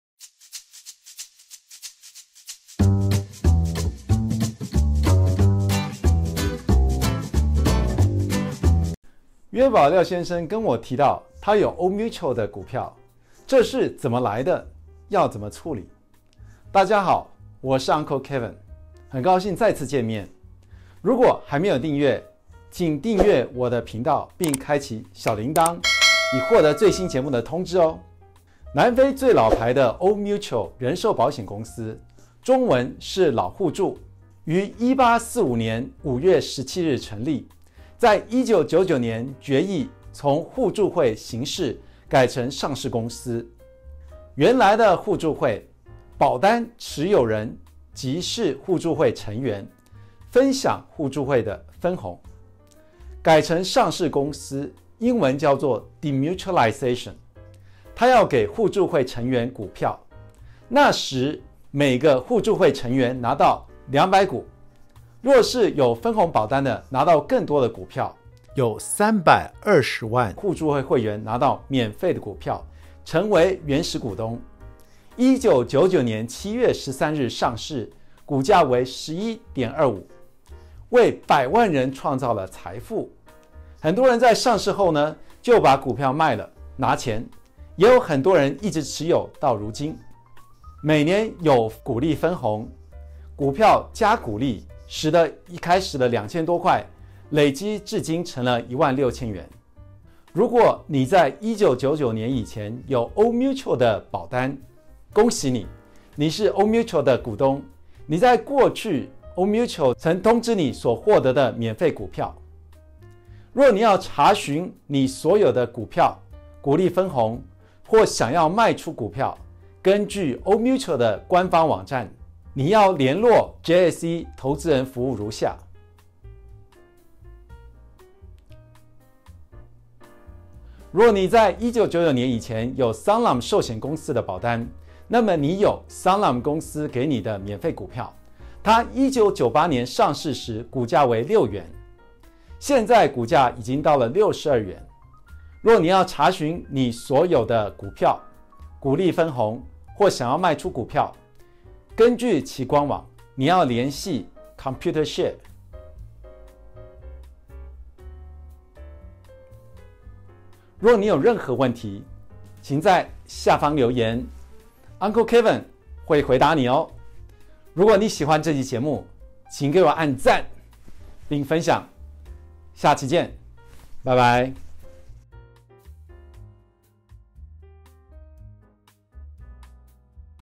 你有1999年以前辦的Old Mutual或Sanlam保單？ E57
Old Mutual及Sanlam分別在1998年及1999年上市，給投保人免費股票。你該怎麼處理這些股票？

请在下面的评论中告诉我们您对此主题的看法。

在微信上找到我们：daberistic